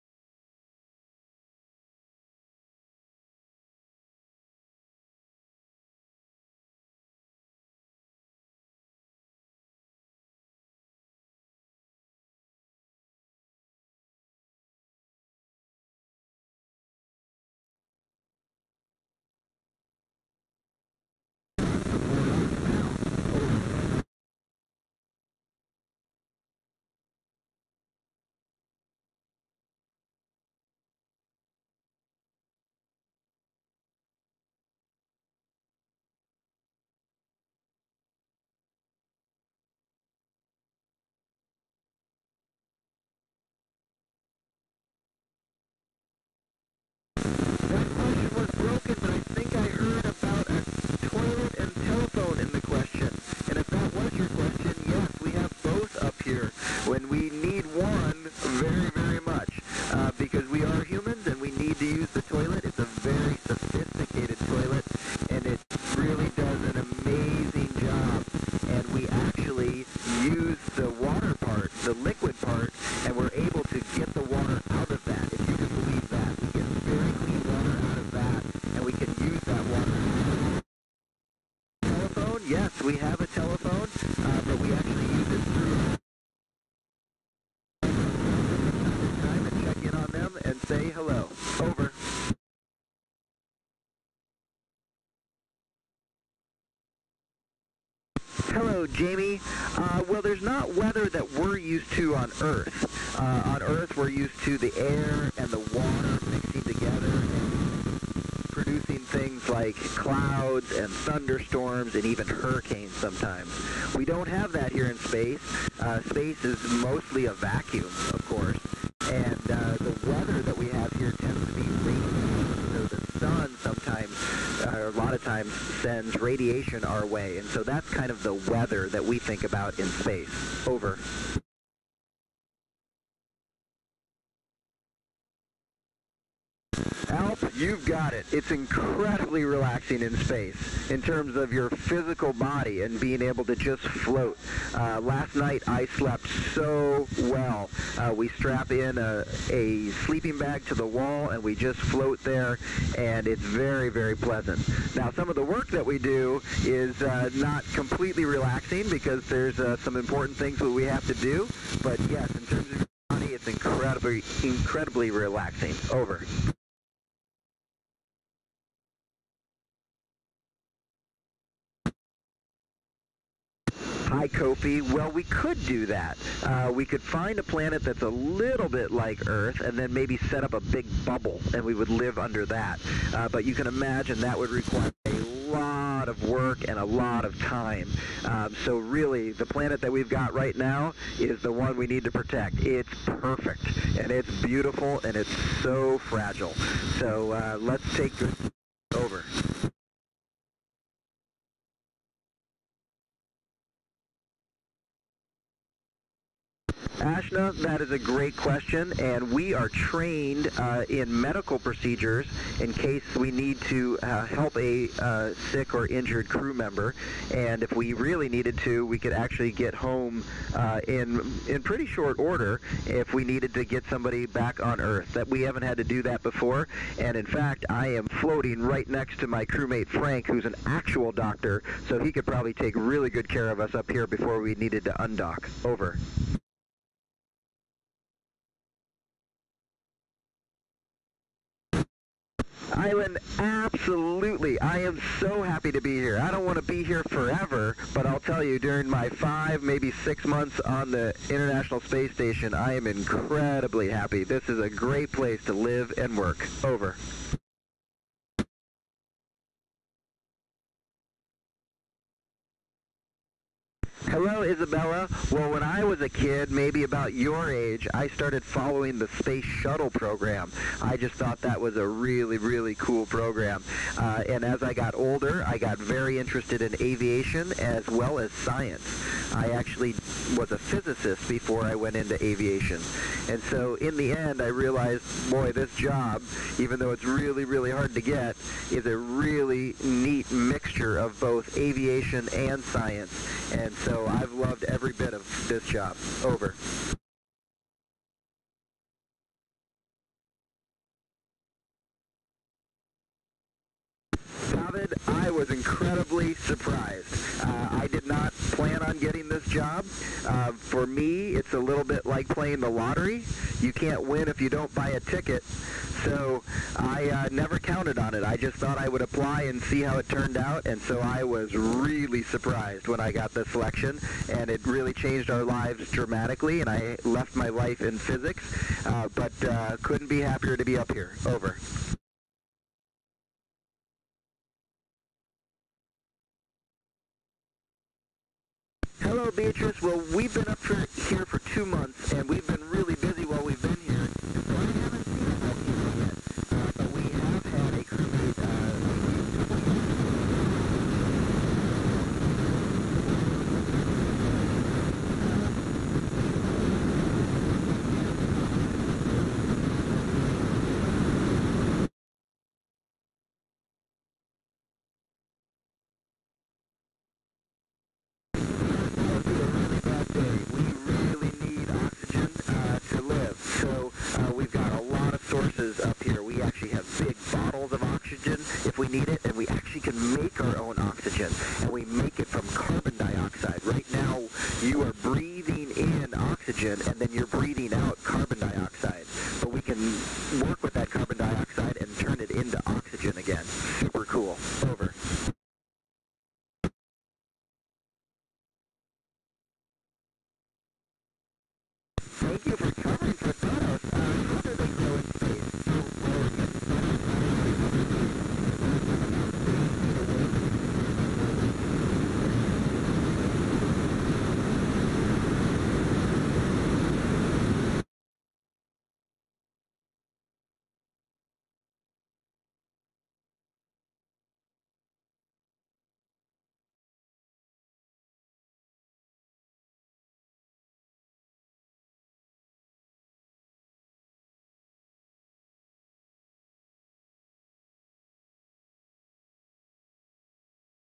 ARISS Contact - British School in the Netherlands, The Hague - 08 December 2022 - 11.09UTC
ARISS Contact - British School in the Netherlands (Junior School Leidschenveen), The Hague, The Netherlands - 08 December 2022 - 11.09UTC

ARISS Contact is Scheduled with Students at

British School in the Netherlands (Junior School Leidschenveen), The Hague, The Netherlands

December 6, 2022—Amateur Radio on the International Space Station (ARISS) has received schedule confirmation for an ARISS radio contact between an astronaut aboard the International Space Station (ISS) and students at the British School in the Netherlands located in The Hague, The Netherlands.  ARISS conducts 60-80 of these special amateur radio contacts each year between students around the globe and crew members with ham radio licenses aboard the ISS.

The British School in the Netherlands (Junior School, Leidschenveen Campus) is an international primary school composed of a student body with ages 4 to 11 years and who represent 43 nationalities.  In preparing for this ARISS contact, the school incorporated space exploration/technology into its STEM curriculum. In addition, students have been working together on space-related projects and age-appropriate activities. Students are learning about radio communications and what it is like to live and work in the ISS and the training required. In their STEM program, first year students have been learning about space related challenges such as building robotic arms like the Canadian-made robotic arm (Canadarm2) installed on the ISS. Students are also being supported by the school’s specialist Science and Technology Lab. Guest expert speakers have also presented different aspects of space-related topics two of which included the space trash (space debris) orbiting Earth to the science-fiction writings of Jules Verne.

This will be a direct contact via Amateur Radio allowing students to ask their questions of Astronaut Josh Cassada, amateur radio call sign KI5CRH. Local Covid-19 protocols are adhered to as applicable for each ARISS contact. The downlink frequency for this contact is 145.800 MHZ and may be heard by listeners that are within the ISS-footprint that also encompasses the relay ground station.

The amateur radio ground station for this contact is in The Hague, The Netherlands. Amateur radio operators using call sign PE1RXJ, will operate the ground station to establish and maintain the ISS connection.

The ARISS radio contact is scheduled for December 8, 2022 at 12:09:54 pm CET (The Hague)

(11:09:54 UTC, 6:09 am EST, 5:09 am CST, 4:09 am MST, 3:09 am PST).

As time allows, students will ask these questions:
1. Why do you need a helmet?
2. What is the moon made of?
3. In space, is there a toilet and can you use a telephone?
4. Is there weather in space?
5. Is it relaxing in space?
6. If this planet is destroyed can we set up on other planets?
7. What would you do if one of the astronauts were sick or injured?
8. Are you happy to live in the space station?
9. What inspired you to be an astronaut?
10. What did you feel when you found out that you were going to space?
11. Have you ever seen a volcano explode from space, and how was it?
12. What happens if the oxygen runs out?
13. How well do plants grow in space?
14. How often do you do space walks?
15. What is the strangest thing you have ever said to mission control?
16. Why does the sun shine on earth but not the rest of space?